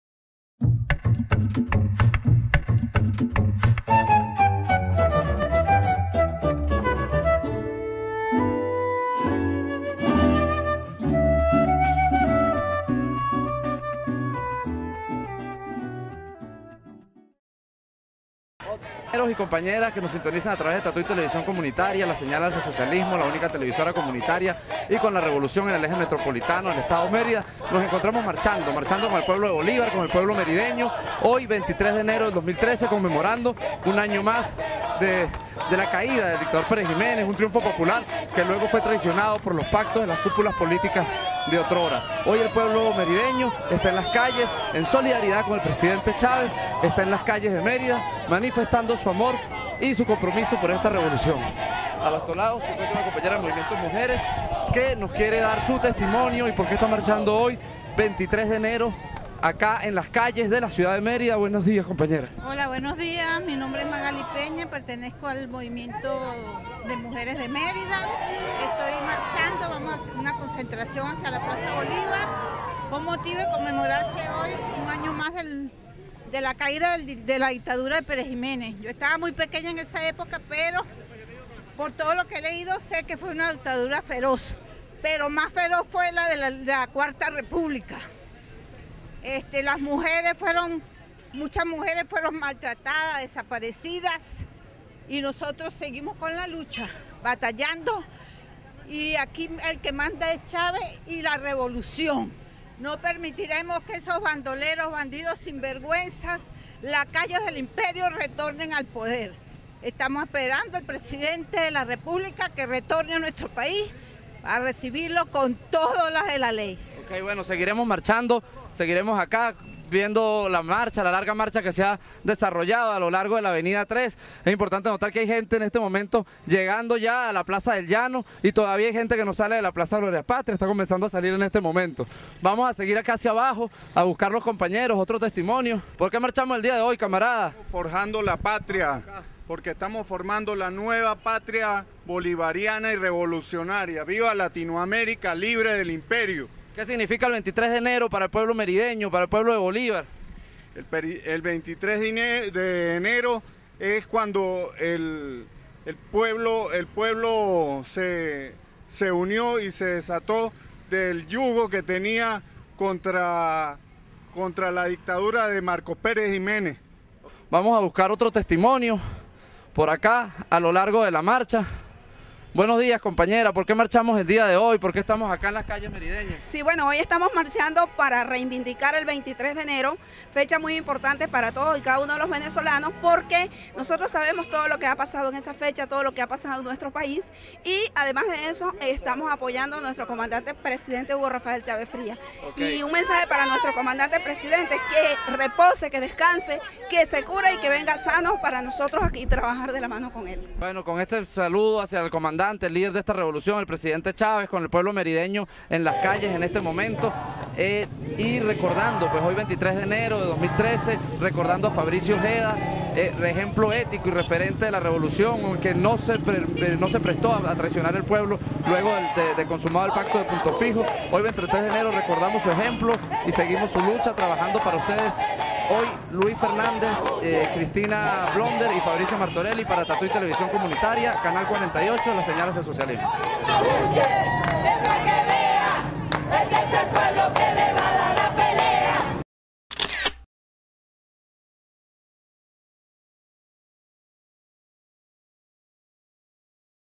Aquí Tatuy Informa: Pueblo Merideño Marcha Conmemorando el 23 de Enero de 1958
En Solidaridad con el Comandante Chávez y Conmemorando el 23 de Enero de 1958, como el triunfo popular que devino en la traición de las cúpulas políticas, El Pueblo merideño salió a las calles, marchando desde la Plaza Glorias Patrias hasta la Plaza Bolívar de la ciudad.
Los presentes manifiestan su apoyo irrestricto al presidente Chávez, anhelando su pronta recuperación, y su convicción firme de no volver al pasado, de no ceder más espacios a quienes hundieron al país durante la Cuarta República.